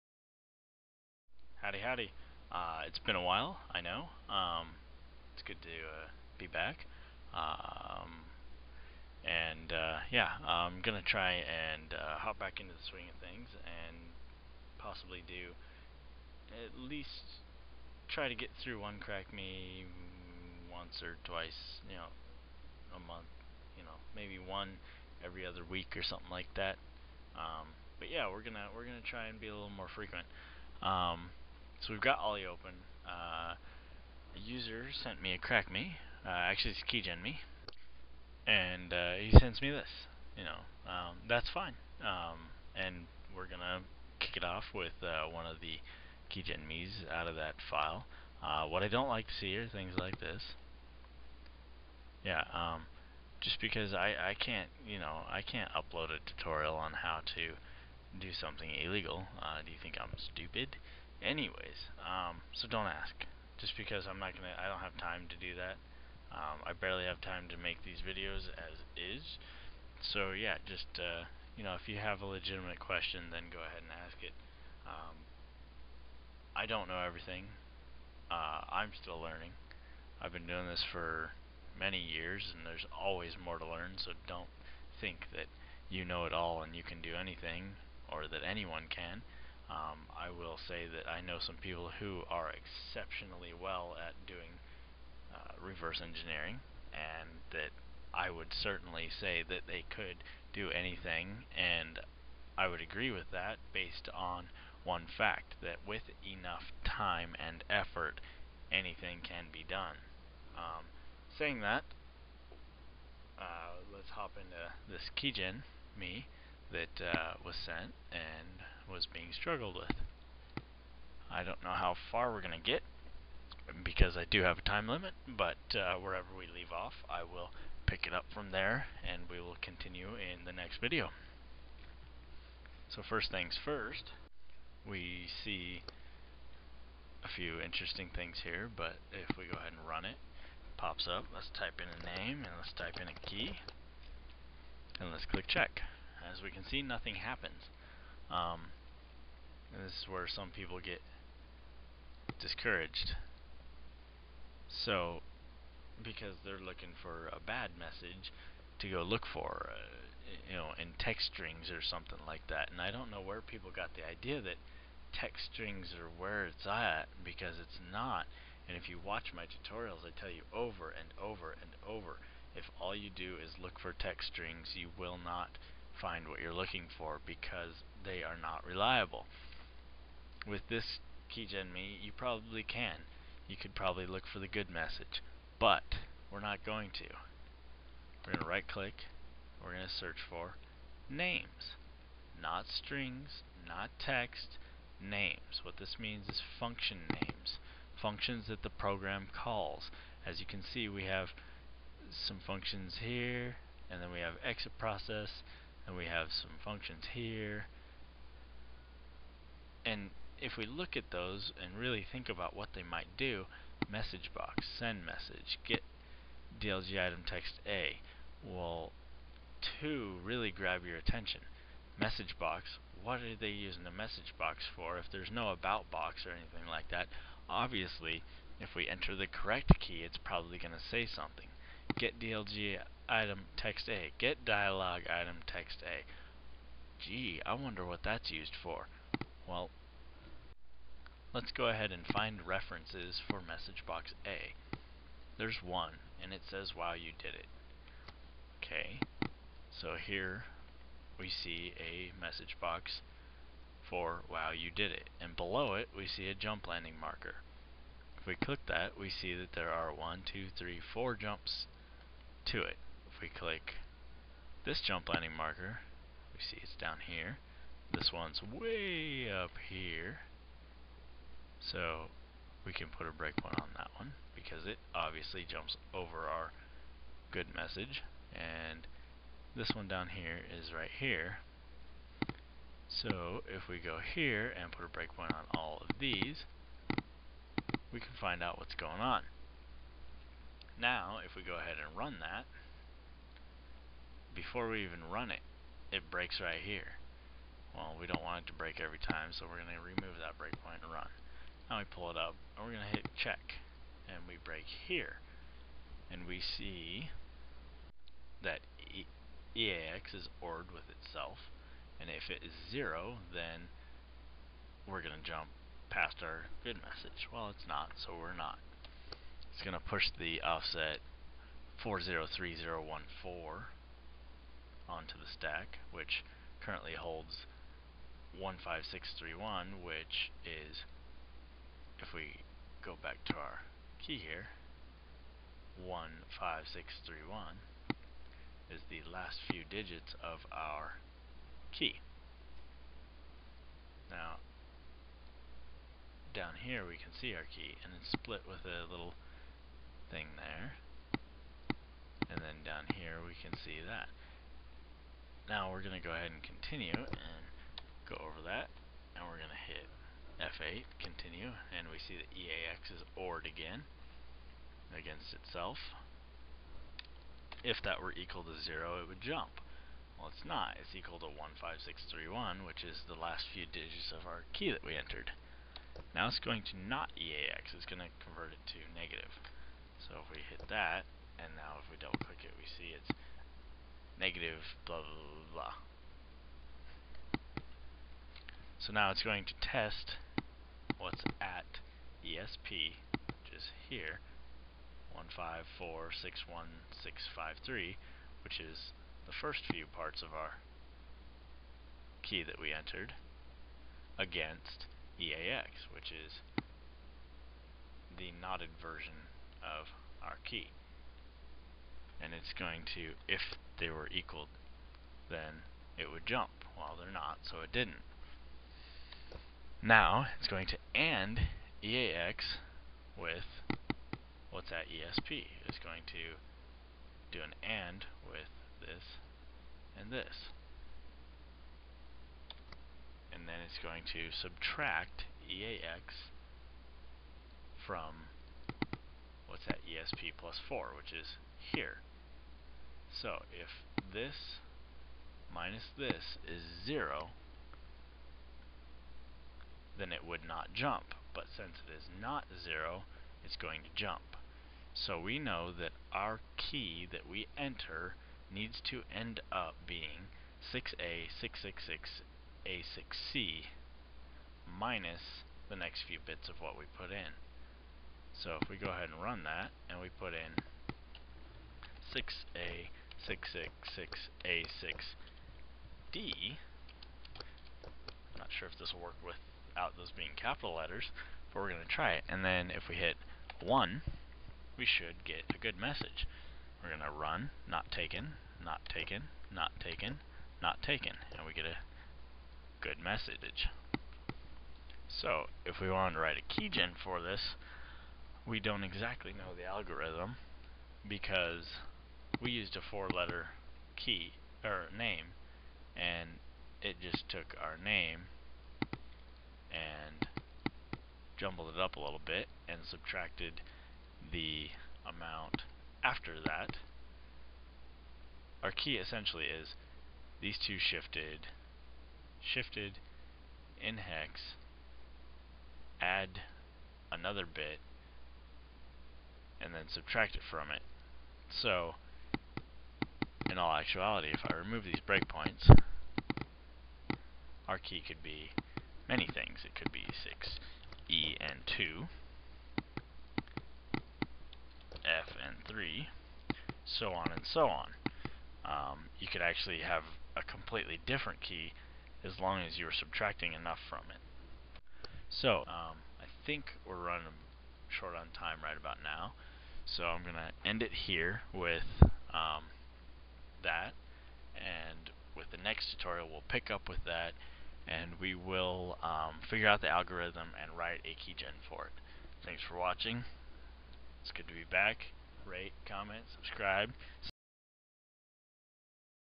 How to crack with ollydbg... IM BACK!
in this video we are going to start fresh going to do some learning, im going to take a look at keygenmes and crackmes that you send me and i am going to document it real-time, as i look at it for the first time you will see it and my mistakes, my steps, what i go through every time i open up a program, nothing will be rehearsed. its all about knowledge and opening yourself up to learn new things!

and folow along :)